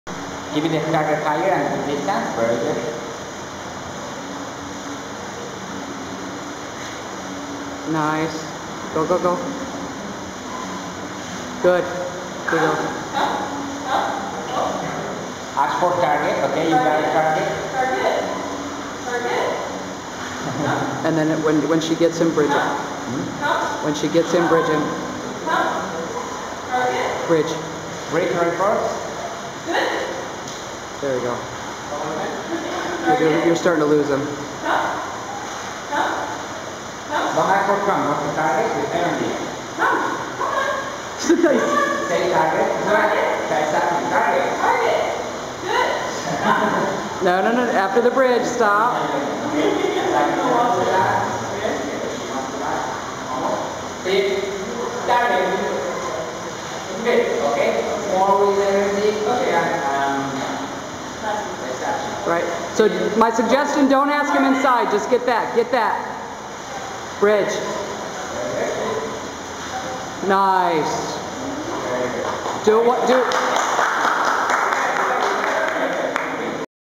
sea lion training.mov
another great workshop learning operant conditioning with hands on learning with marine mammals. a one year old sea lion "popeye" teaches students about training and how important body language and tones of verbal cues can be. fun!